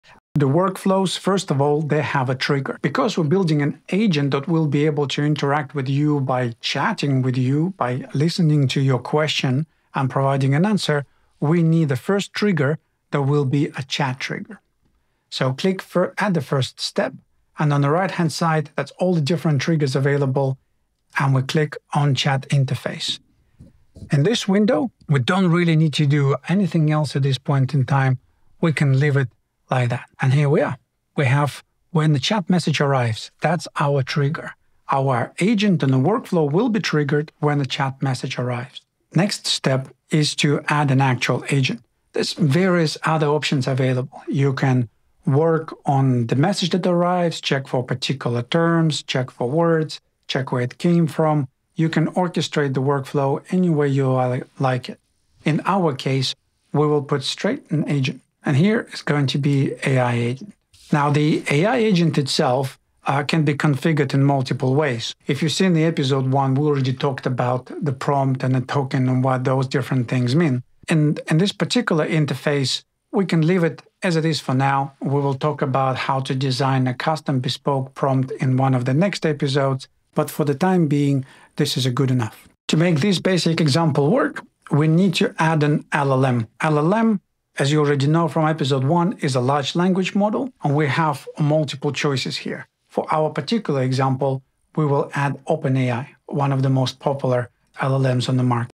Build an AI Chatbot with n8n: Easy Workflow Guide
Learn how to build a workflow in n8n, starting with a chat trigger and integrating an AI agent. We'll cover setting up your chat interface, configuring the AI agent, and adding an LLM like OpenAI. Follow along to create your own interactive AI assistant!